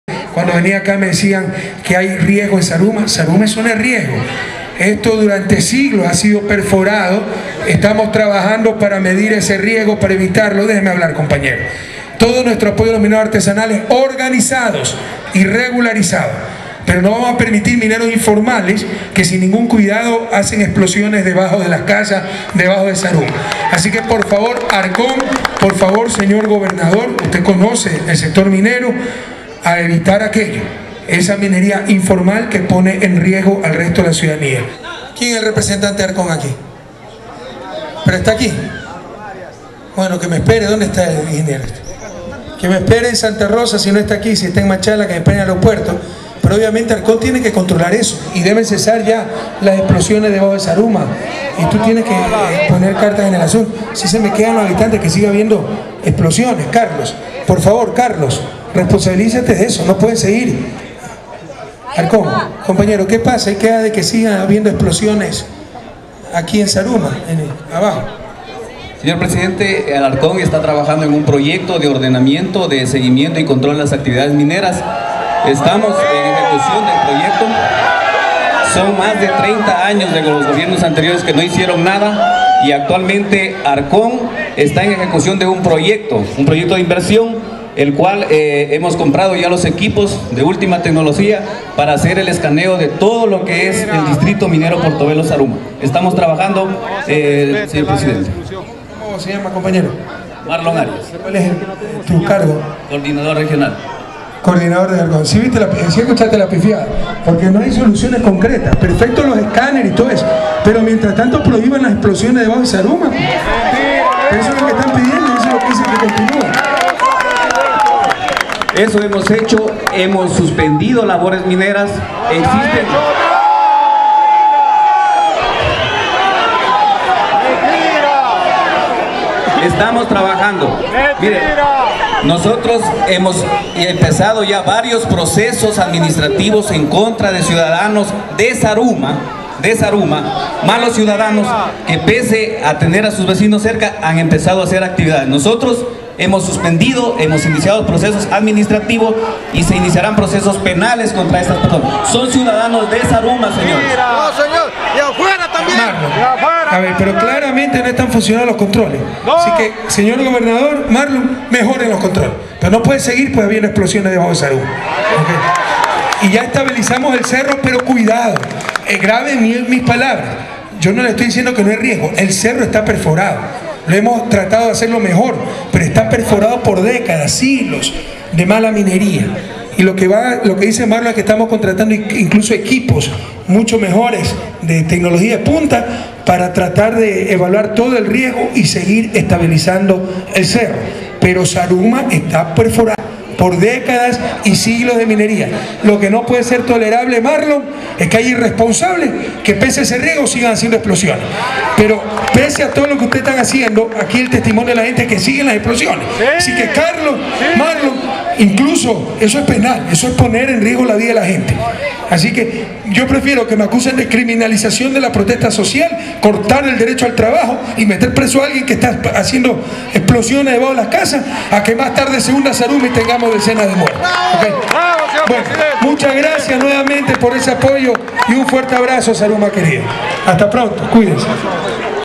En Zaruma Correa pide mayor control a funcionario de ARCOM
El presidente Correa llamo la atención al coordinador regional de ARCOM en El Oro, por la falta de control a las explosiones mineras en la ciudad de Zaruma